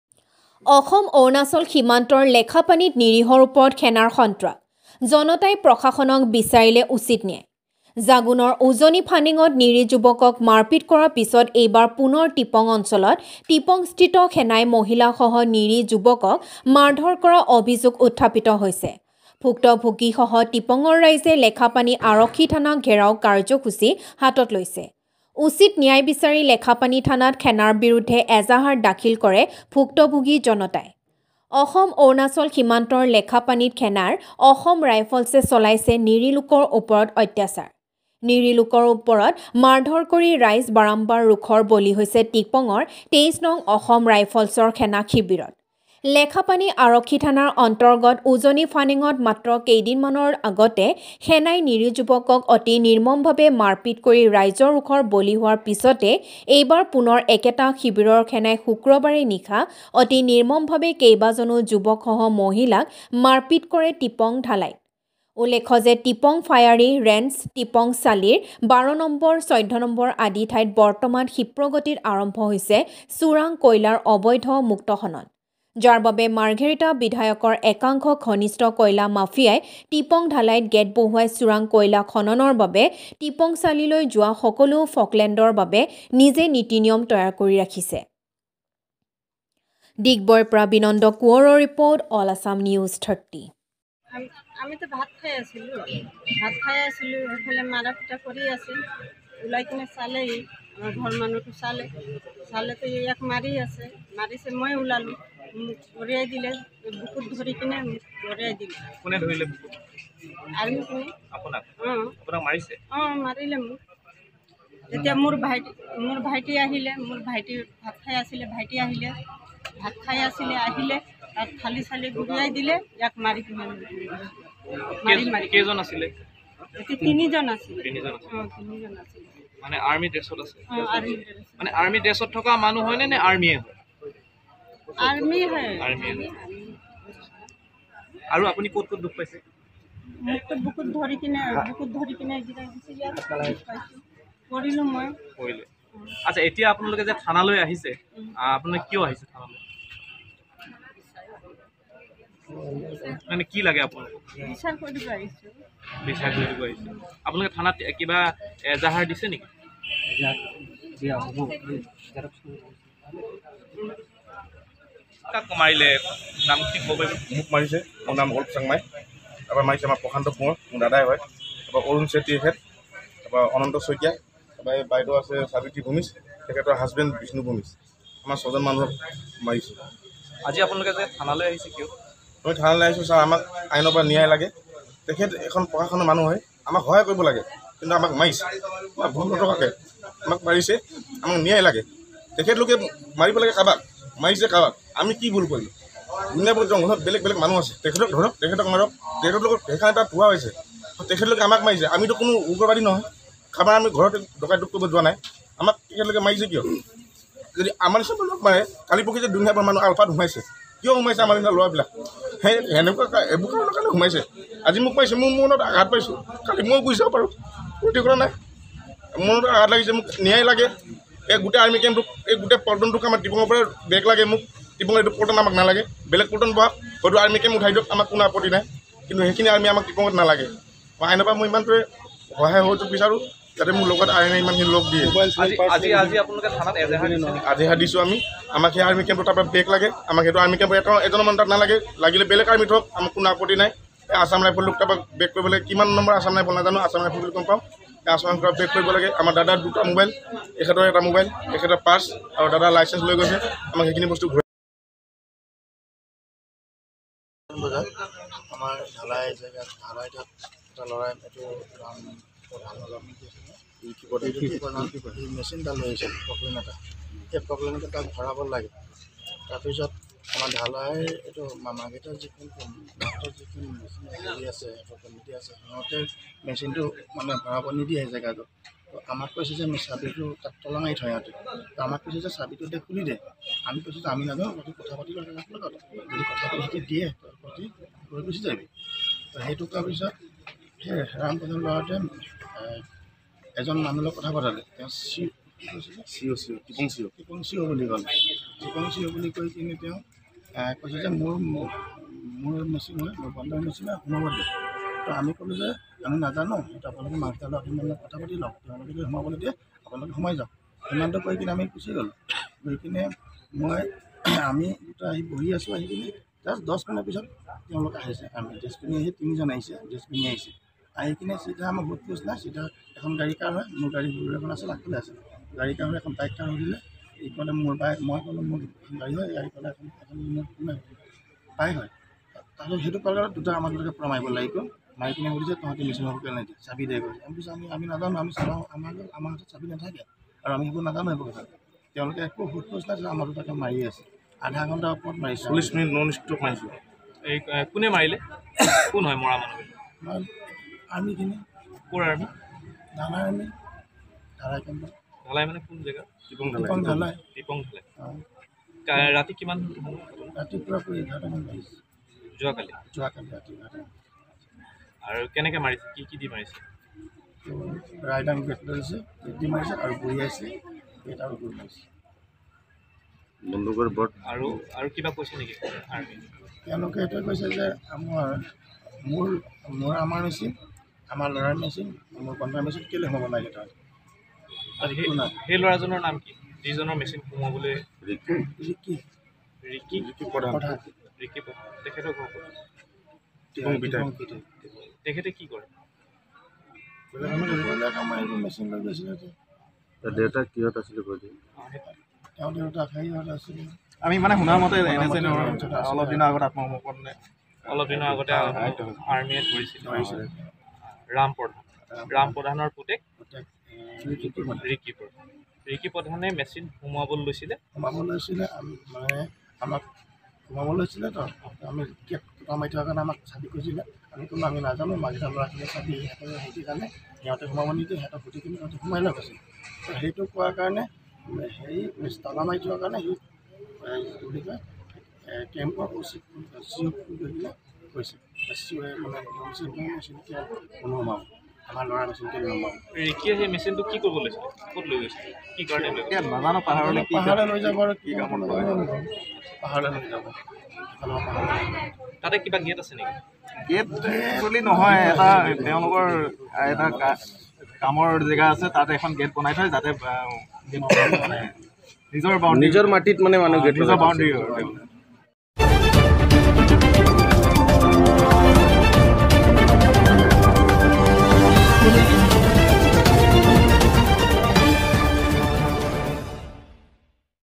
Digboi News | অসম-অৰুণাচল সীমান্তৰ লেখাপানীত নিৰিহৰ ওপৰত সেনাৰ সন্ত্ৰাস
অসম-অৰুণাচল সীমান্তৰ লেখাপানীত নিৰিহৰ ওপৰত সেনাৰ সন্ত্ৰাস